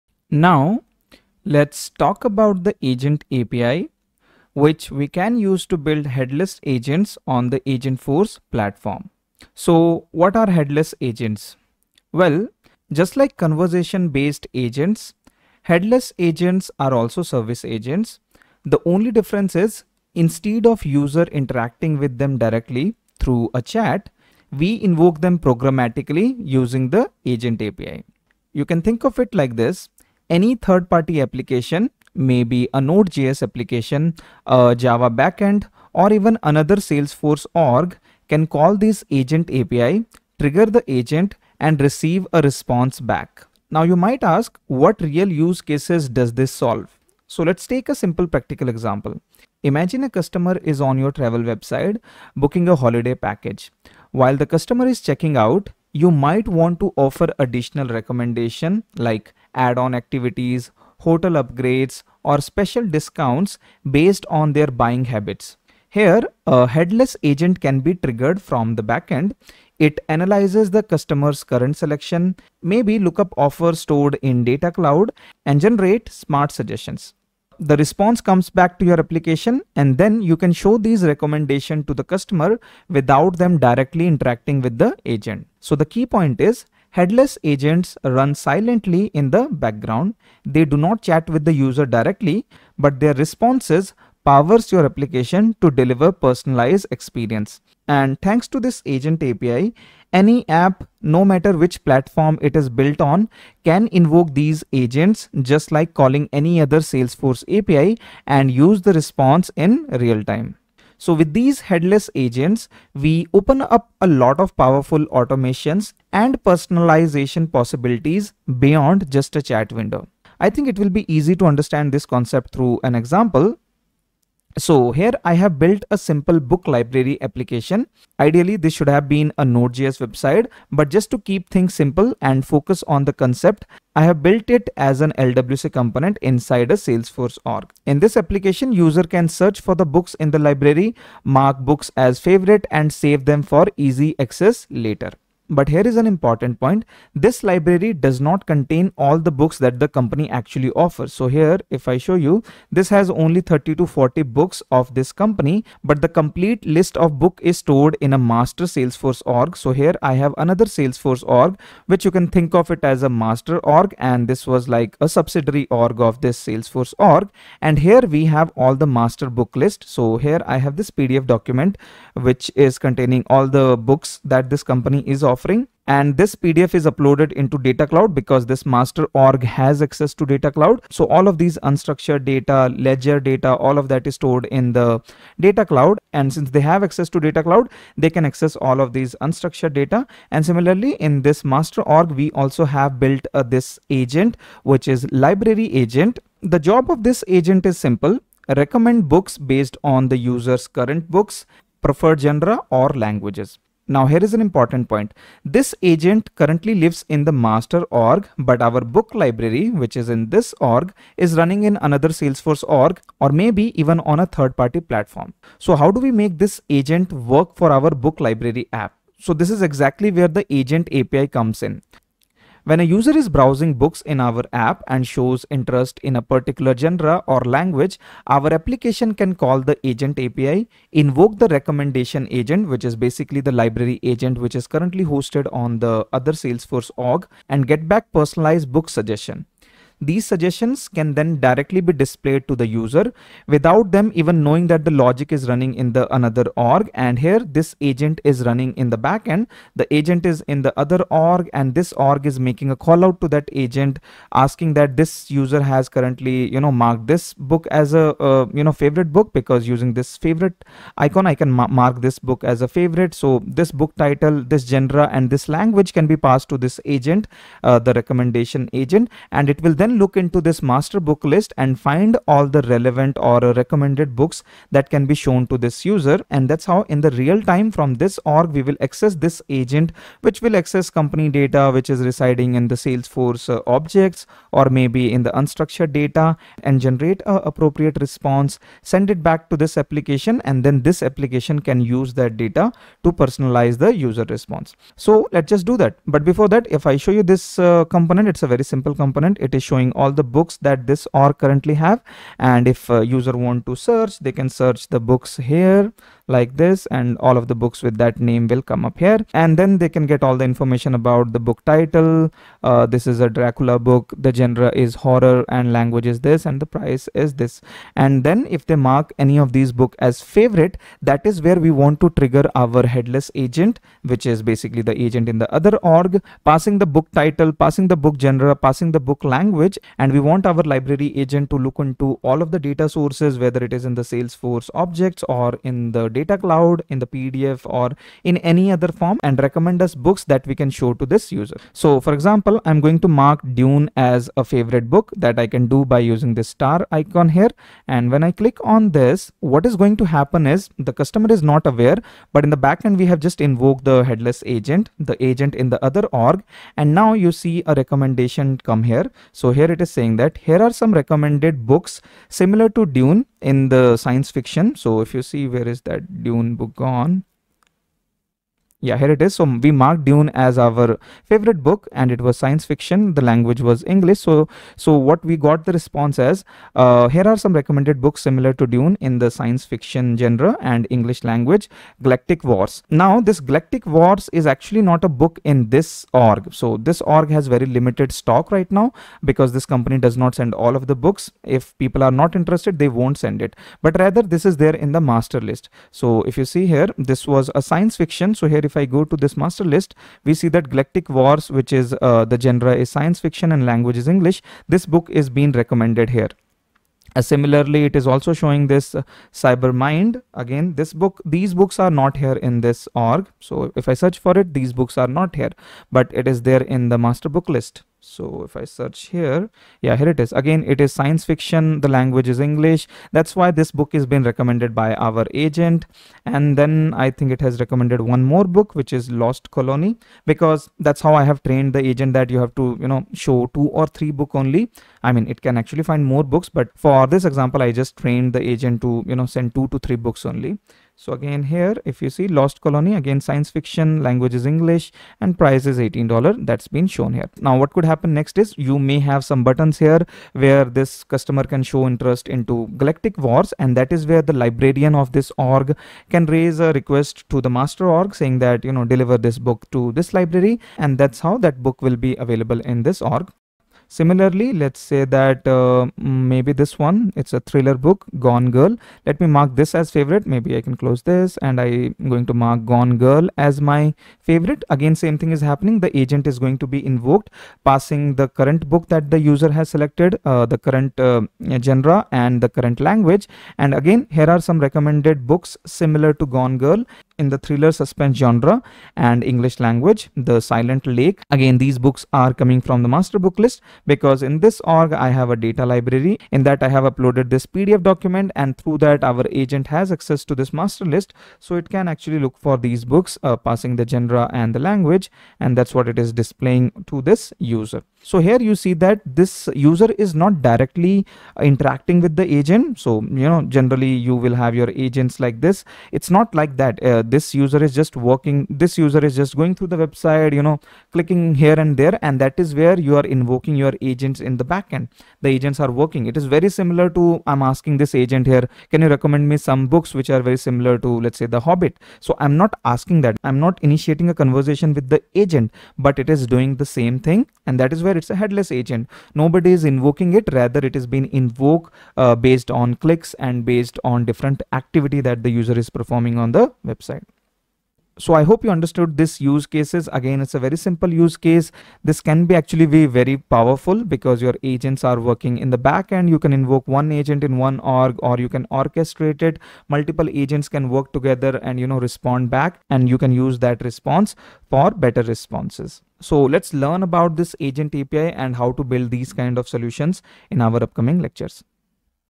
Lecture 1 : Headless Agents in Salesforce Using Agent API | Agentforce | Demo
In this video, we explore how to build Headless Agents in Salesforce using the Agent API. Unlike traditional chat-based agents, headless agents are triggered programmatically and run silently in the background.

🔹 What You’ll Learn:

What is a headless agent?

When and why to use the Agent API

A practical use case using a Book Library app

Triggering agents across orgs via API

How to personalize user experience without direct interaction

Leveraging Data Cloud and unstructured data

🔹 Use Case Highlight:
We simulate a multi-org scenario where a recommendation agent is hosted in a master org, and invoked from a different org based on user activity in a book library application. The headless agent responds with relevant book recommendations in real time—without any chat interface.

🔹 Technologies Used:

Salesforce Agentforce

Agent API

Data Cloud

LWC

Multi-org API Integration

📌 Ideal for: Salesforce Developers, Architects, and Consultants exploring real-world automation with Agentforce and API-driven solutions.

🧠 Learn how to bring advanced personalization to your apps with just API calls!